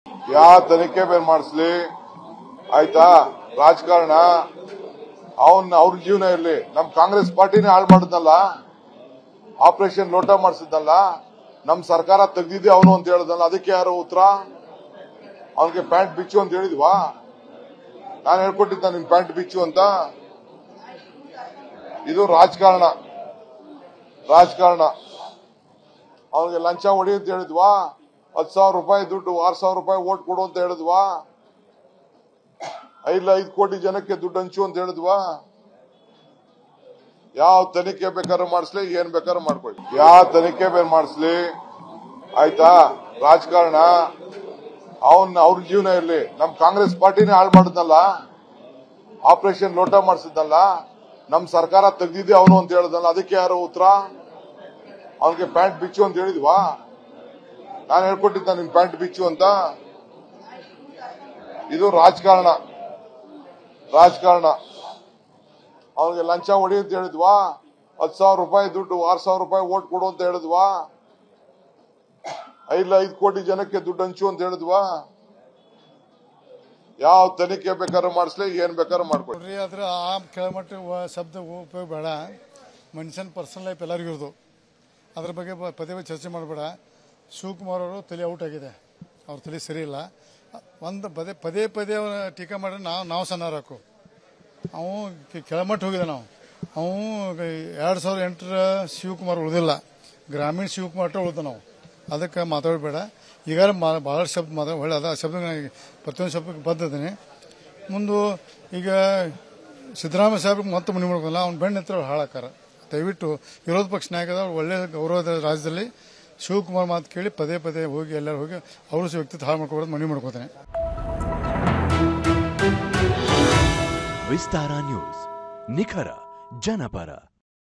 ಕೆಳಮಟ್ಟದ ಪದ ಬಳಕೆ ಬೇಡ.. ಸಾಹುಕಾರ್ ಎಚ್ಚರಿಕೆ | D K Shivkumar | Ramesh Jarakiholi | Vistara News Kannada
ಕೆಪಿಸಿಸಿ ಅಧ್ಯಕ್ಷ ಡಿ.ಕೆ.ಶಿವಕುಮಾರ್ ಹೇಳಿಕೆಗೆ ಬೆಳಗಾವಿಯಲ್ಲಿ ಮಾಜಿ ಸಚಿವ ರಮೇಶ್ ಜಾರಕಿಹೊಳಿ ತಿರುಗೇಟು ನೀಡಿದ್ದಾರೆ.